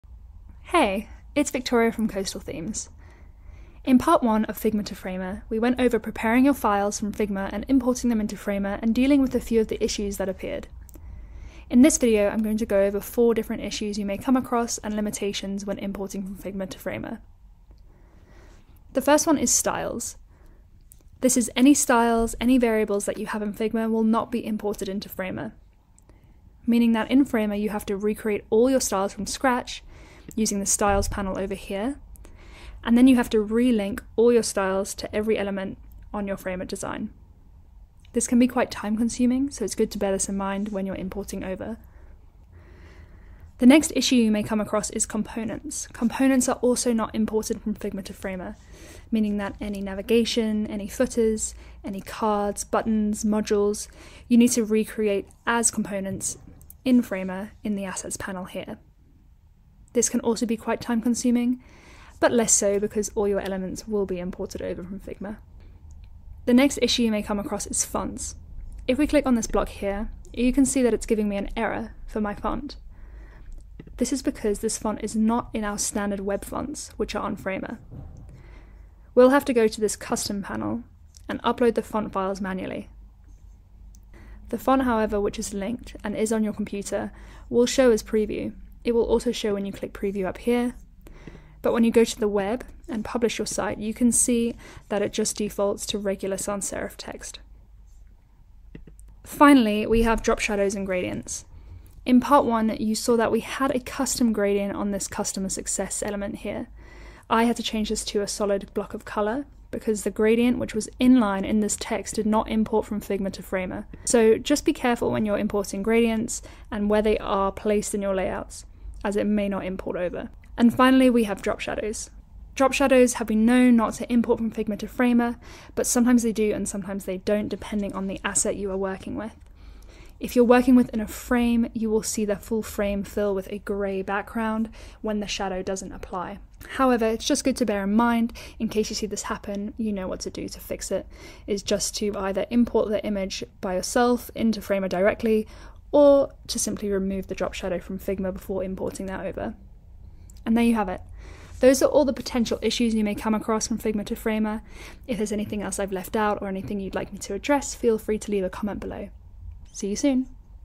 Figma to Framer Part 2: Importing Components, Fonts & Drop-Shadows
In this video, we'll show you how to fix some common issues when copying your @Figma designs into @Framer

We look at styles, components, fonts, drop-shadows & gradients.

Is there a specific topic that you'd like us to show you on @Framer ? Get in touch with us!